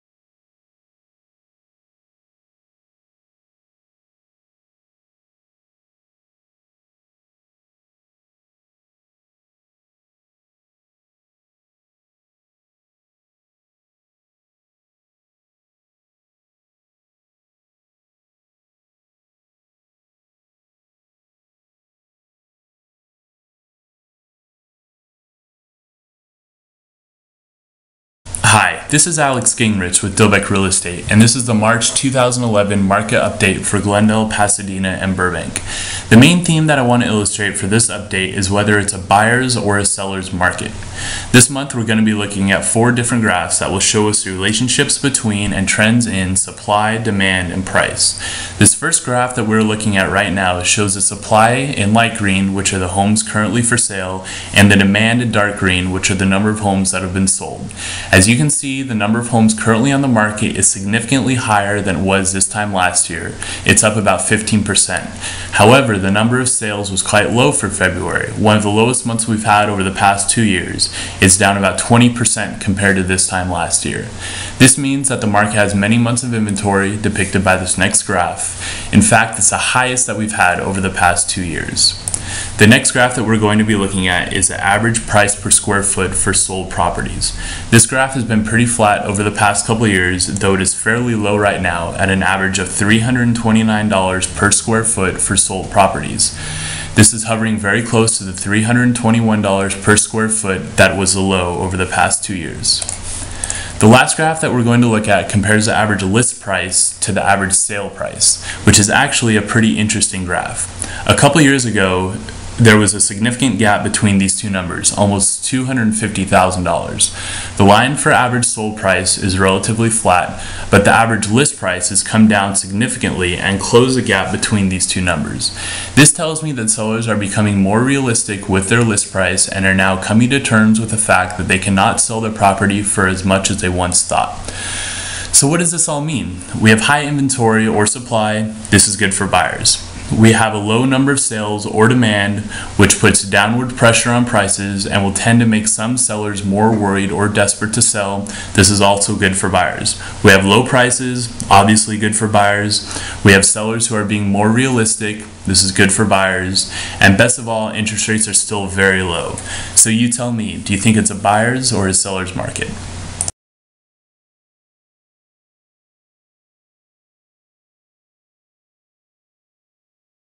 March 2011 Market Update - Glendale CA, Pasadena, Burbank
Is it a Buyer's or a Seller's market? This is a real estate market update for March 2011 for Glendale CA, Pasadena, and Burbank. It discusses the number of sales, number of listings, price per square foot, and the months of inventory to illustrate that it is currently a buyer's market.